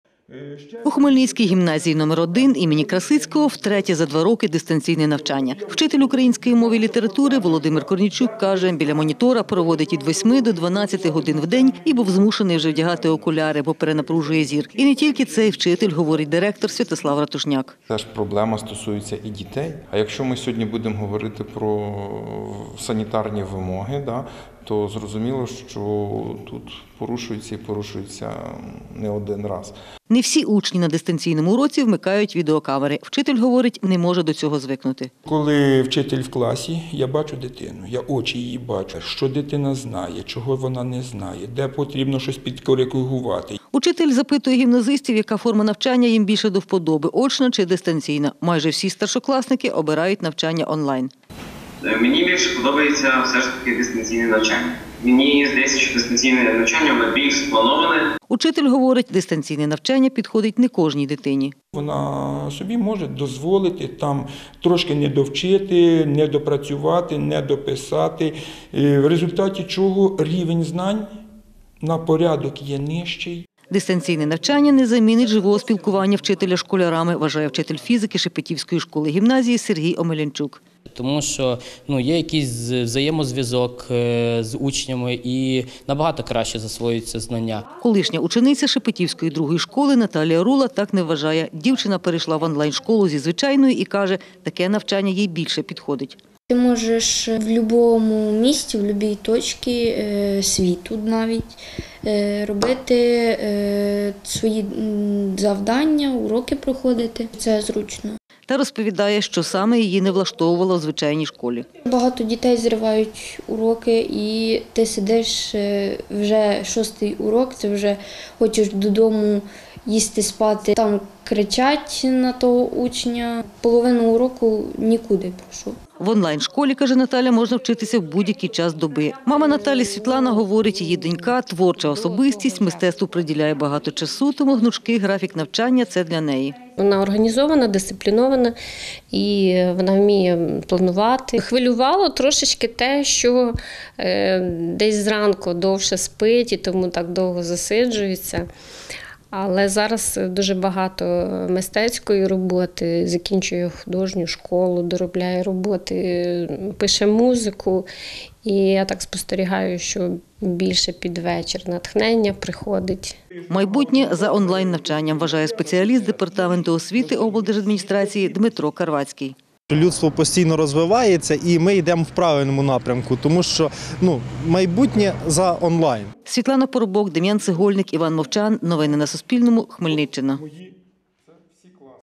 Шепетівчанка на постійній основі вирішила дистанційно опановувати шкільну програму вдома
Не повертатися до навчання у школі, а продовжити дистанційно опановувати шкільну програму вдома, на постійній основі вирішила шепетівчанка Наталія Рула. Каже, дистанційне навчання, яке стало вимушеним кроком в умовах пандемії, їй сподобалось. Згадує, минулого року вона вступила в Київську онлайн – школу, навчалася і там, і в Шепетівській другій школі.

 та натисніть дзвіночок, щоб отримати сповіщення про нові відео та трансляції